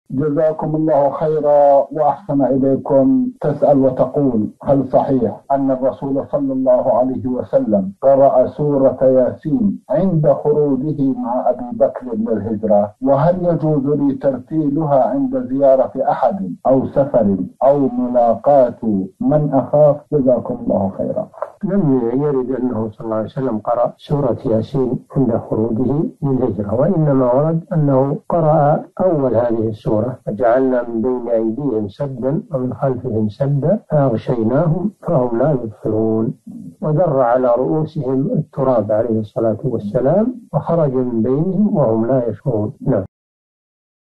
هل صحيح أن الرسول ﷺ قرأ سورة يس عند خروجه مع أبي بكر للهجرة، وهل يجوز ترتيلها عند زيارة أحد؟
نص السؤال: هل صحيح أن الرسول ﷺ قرأ سورة يس عند خروجه مع أبي بكر للهجرة، وهل يجوز لي ترتيلها عند زيارة أحدٍ أو سفرٍ أو ملاقاة من أخاف؟
السيرة الذاتية :
للاشتراك في قناة الشيخ صالح الفوزان

رئيس الفريق
لمشروع كبار العلماء